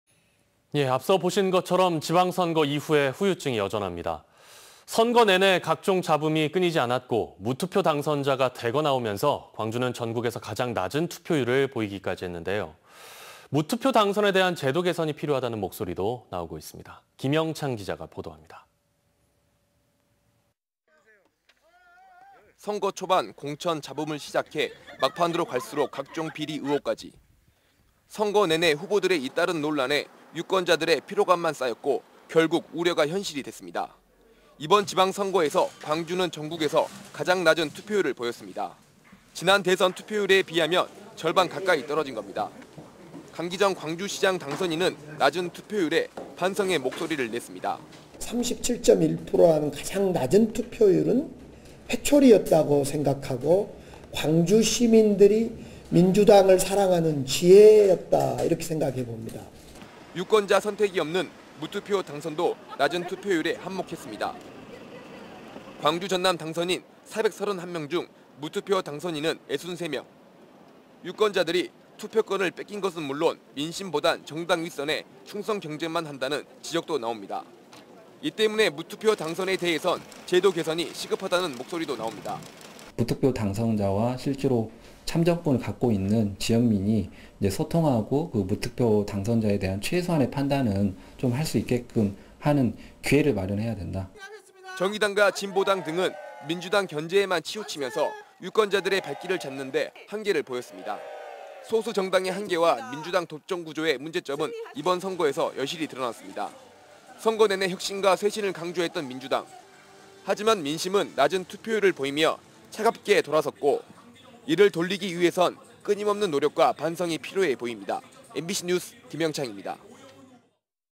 낮은 투표율.. '무투표 당선' 개선 필요 -R (220605일/뉴스데스크)
◀ＶＣＲ▶

선거 초반 공천 잡음을 시작해
막판으로 갈 수록 각종 비리 의혹까지...

선거 내내 후보들의 잇따른 논란에
유권자들의 피로감만 쌓였고,
결국 우려가 현실이 됐습니다

이번 지방선거에서 광주는
전국에서 가장 낮은 투표율을 보였습니다.

지난 대선 투표율에 비하면
절반 가까이 떨어진 겁니다.

강기정 광주시장 당선인은
낮은 투표율에
반성의 목소리를 냈습니다.

◀ＳＹＮ▶강기정 광주시장 당선인 (지난 2일)
37%라는 가장 낮은 투표율은
회초리였다고 생각하고
광주시민들이 민주당을 사랑하다는
지혜였다고 생각합니다.

유권자 선택이 없는
무투표 당선도
낮은 투표율에 한 몫 했습니다.

광주전남 당선인 431명 중
무투표 당선인은 63명.

유권자들이 투표권을 뺏긴 것은 물론
민심보단,
정당 윗선에 충성 경쟁만 한다는
지적도 나옵니다.

이 때문에 무투표 당선에 대해서
제도개선이 시급하다는 목소리도 나옵니다.

◀ＳＹＮ▶조선익 / 참여자치21 공동대표
"무투표 당선자와 실제로 참정권을
갖고 있는 지역민이 소통하고 무투표 당선자에 대한
최소한의 판단은 할수 있게끔 하는 기회를 마련해야 된다.

정의당과 진보당 등은
민주당 견제에만 치우치면서,
유권자들의 발길을
잡는데 한계를 보였습니다.

소수정당의 한계와
민주당 독점 구조의 문제점은
이번 선거에서 여실히 드러났습니다.

선거 내내 혁신과 쇄신을 강조했던 민주당.

하지만 민심은 낮은 투표율을 보이며
차갑게 돌아섰고,
이를 돌리기 위해선 끊임없는 노력과 반성이
필요해 보입니다.

mbc뉴스 김영창 입니다.

◀ＥＮＤ▶